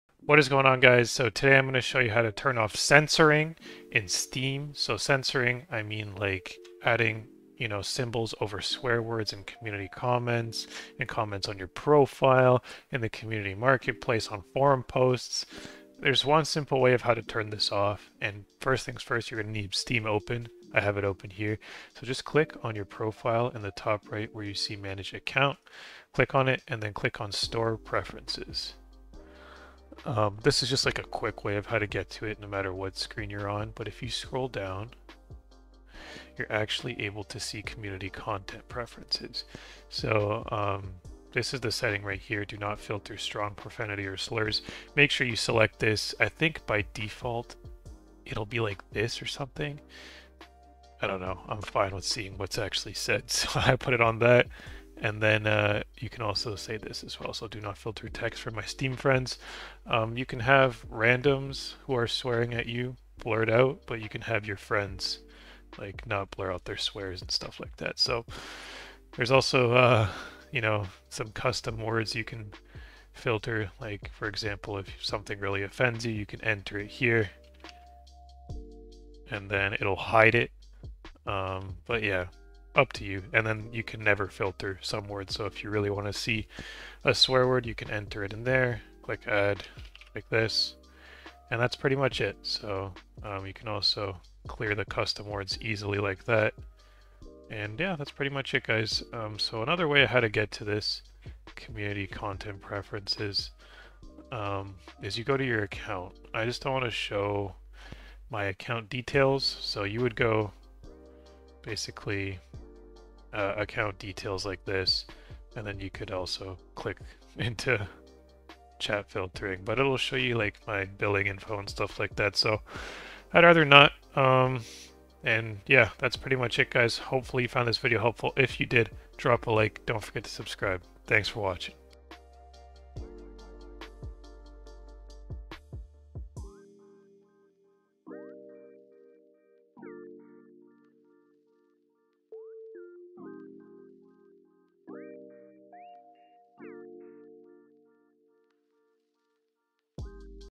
How to Turn Off Censoring Words in Steam!
How to turn off/on censoring words in Steam! (Swear words, custom words, etc..)